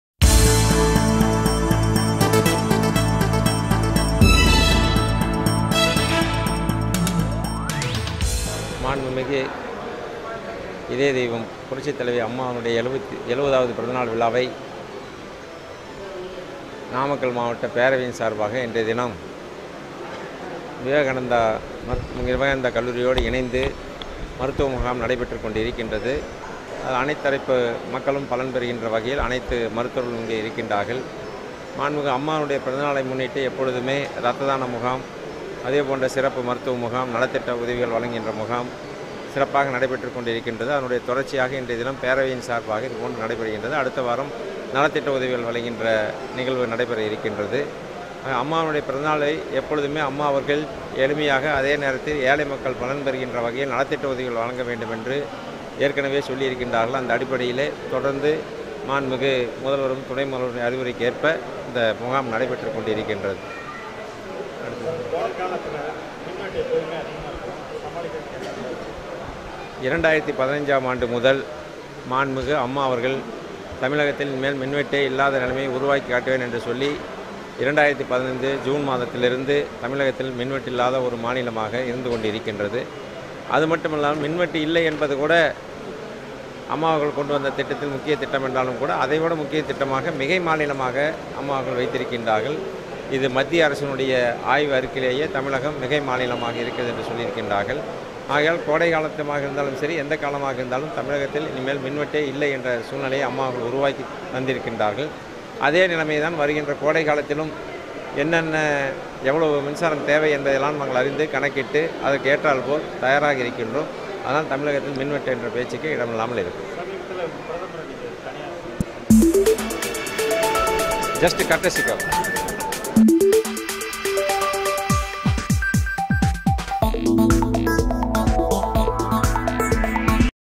கோடையில் மின் வெட்டு வராது – அமைச்சர் தங்கமணி பேட்டி.
தமிழகம் மின் மிகை மாநிலமாக இருப்பதால், கோடை காலத்தில் மின்வெட்டு என்ற பேச்சிற்கே இடம் இல்லை என அமைச்சர் தங்கமணி திருச்செங்கோட்டில் தெரிவித்தார்.